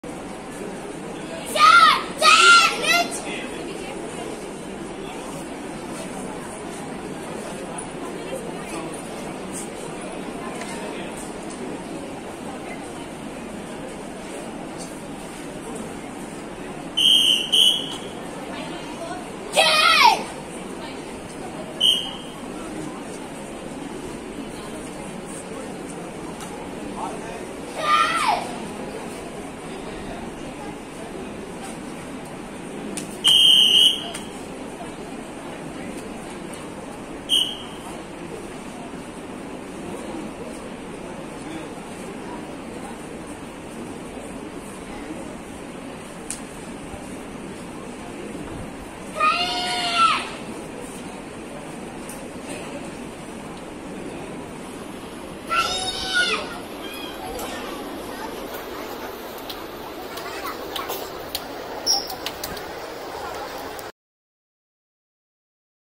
Ithu epdi iruku ?
Golden performance of Maagi at 31st district karate championship 🥇